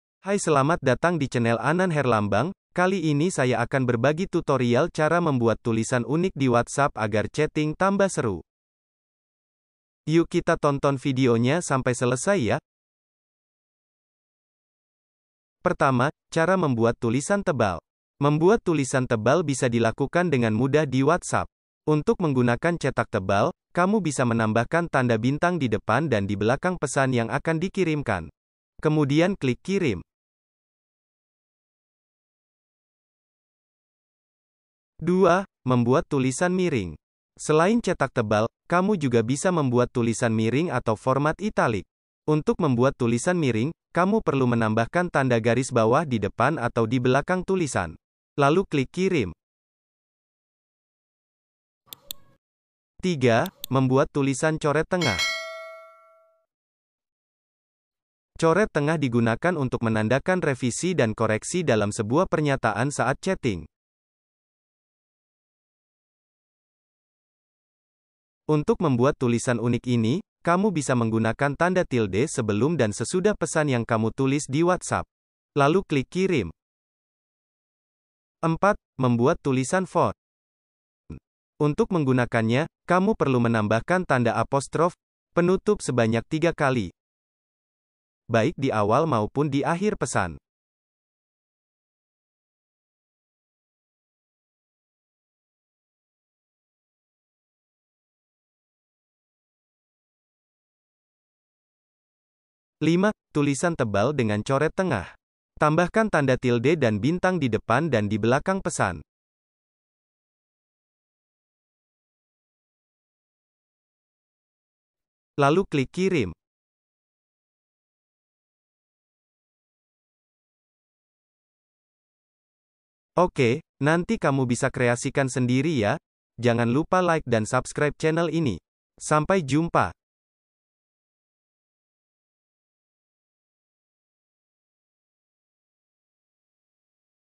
Tutorial Membuat Tulisan Unik Di Whatsapp
Hayy semua, Jumpa kembali dengan saya di Channel Anan Herlambang, Kali ini saya akan berbagi tentang cara membuat tulisan unik di whatsapp yang mungkin belum pernah kalian ketahui.
Sebenarnya sangat mudah cara untuk membuat tulisan unik di WA, Nah kalian bisa segera mempraktekkannya setelah menonton vidio ini yaa..
Yuk kita tonton saja Vidio trik membuat tulisan unik di WA.